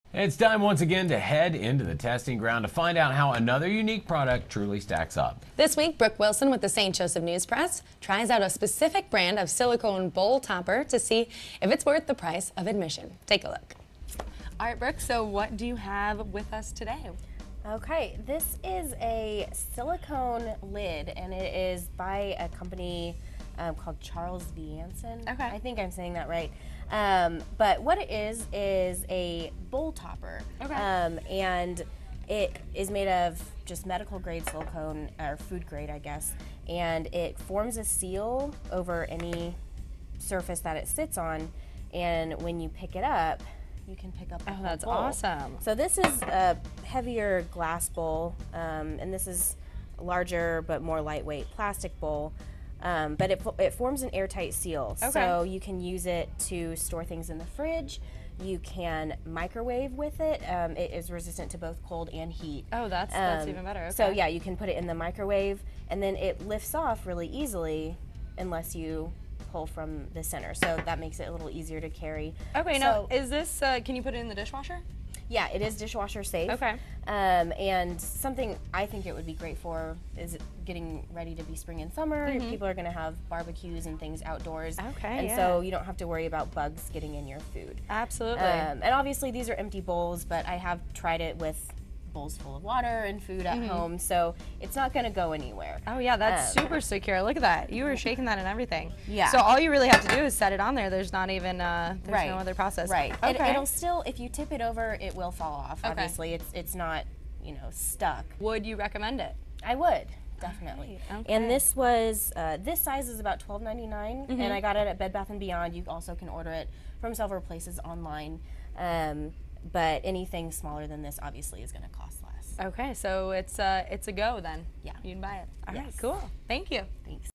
The Testing Ground - Charles Viancin Silicone Suction Lid
Brooke Wilson from the St. Joseph News-Press reviews this pretty alternative to plastic wrap.